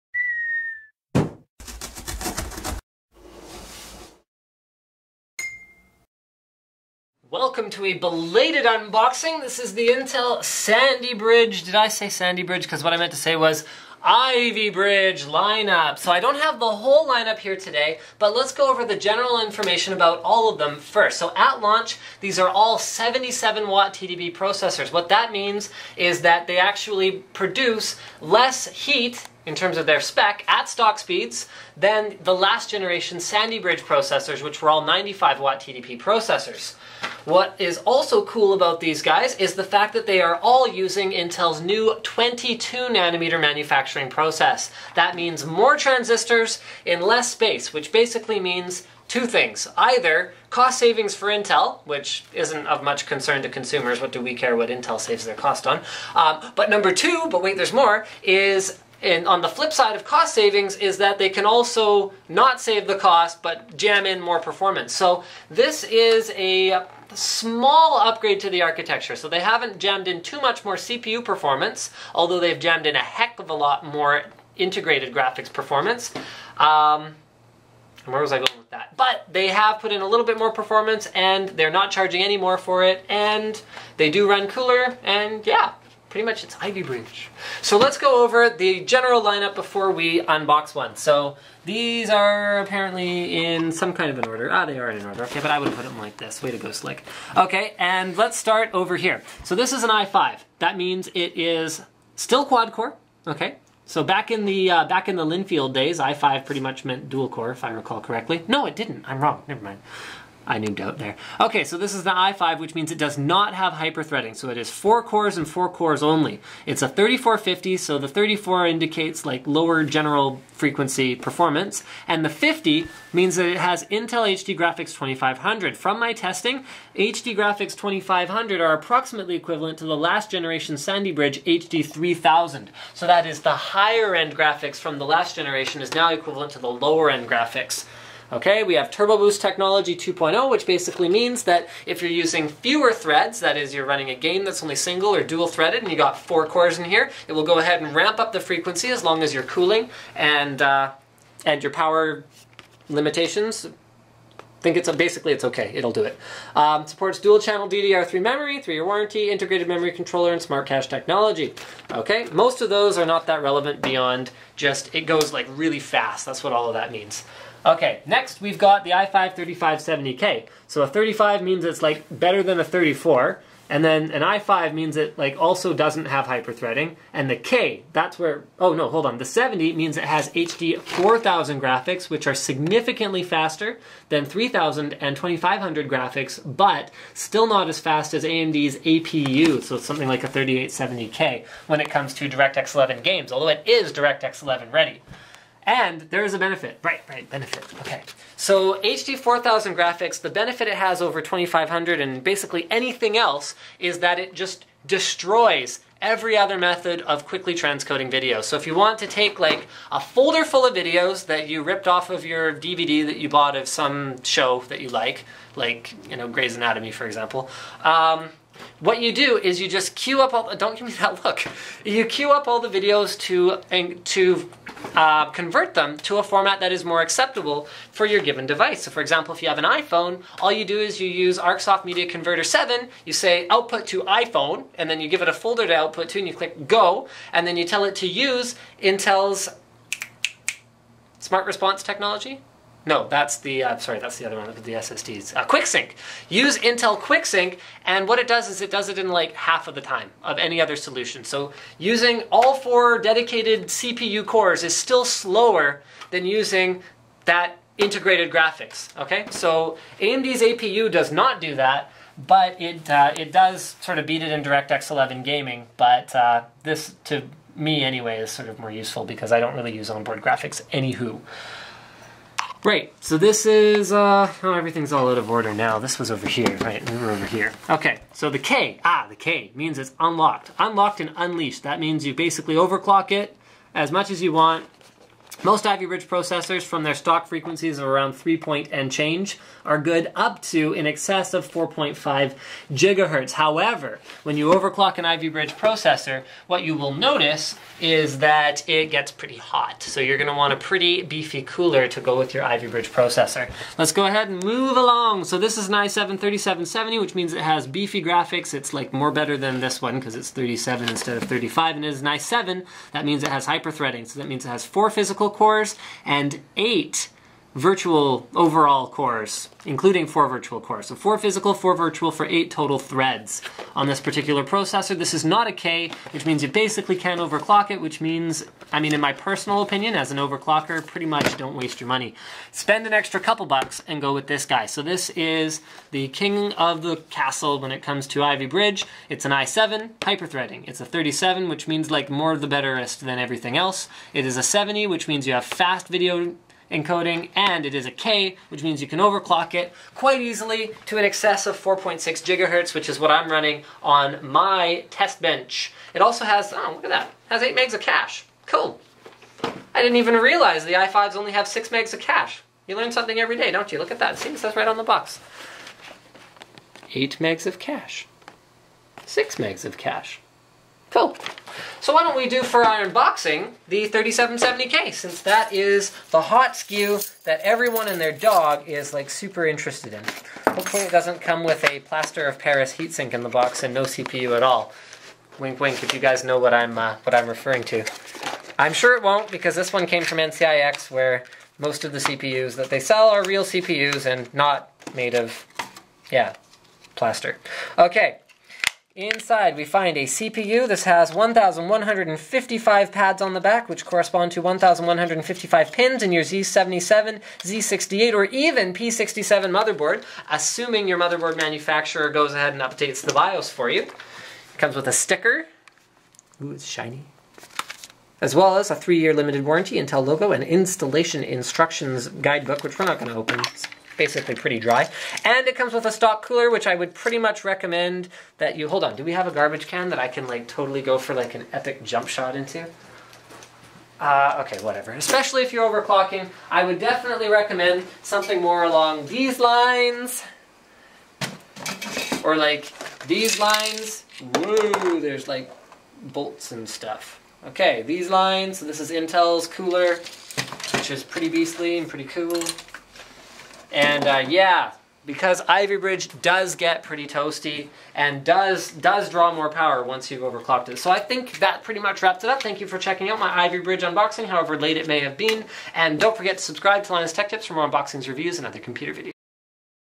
Intel 3770K 3570K Ivy Bridge 3rd Generation Core CPU Unboxing & First Look Linus Tech Tips
Sorry this is late guys, but it's finally done! Ivy Bridge unboxings AWAY! Includes 4 different CPUs, the 3770K, the 3570K, the 3770, and the 3450. I also explain what the numbers and letters mean.